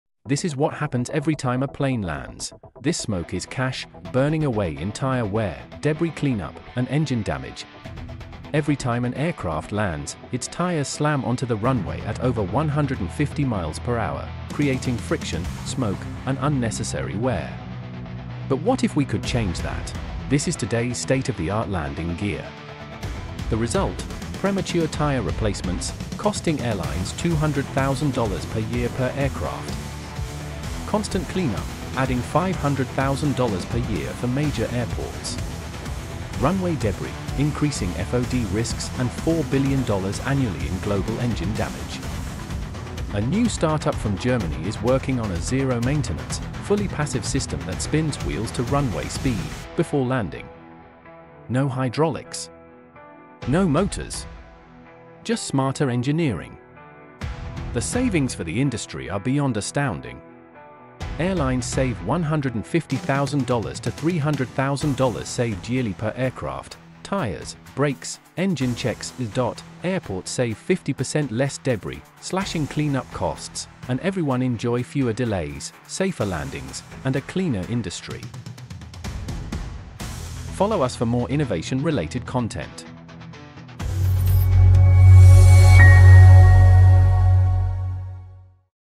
Airlines Are Wasting $200K Per Plane on THIS (The Fix Will Blow Your Mind)
✈️ Every time a plane lands, it burns $200,000/year in hidden costs—until now.

In this video, we reveal the revolutionary passive wheel-sync system that:
✅ Eliminates 70% of tire wear
✅ Requires ZERO maintenance or power
✅ Saves airlines $150K–300K per plane annually
✅ Reduces runway debris & engine damage

🔧 How it works: Ultra-lightweight (just +7kg/wheel), auto-syncs to runway speed, and even acts as an emergency power backup.

📊 For airlines, airports, and aviation engineers: This isn’t just innovation—it’s free money waiting to be claimed.

📌 CHAPTERS:

00:00 - Intro
00:07 - Why do airplane tires smoke
00:26 - The tires costs for airlines
00:30 - The runways cleaning costs for airports
00:38 - The FOB risks and costs for debris damage
00:43 - A mew startup from comes with an ingenious new idea
00:51 - The main benefits of the invention
00:47 - The yearly savings for the airlines
01:12 - What everyone enjoy at the end

🔗 Want the full technical specs? Leave a comment or write a message

📢 Tag an airline exec or aviation engineer who needs to see this!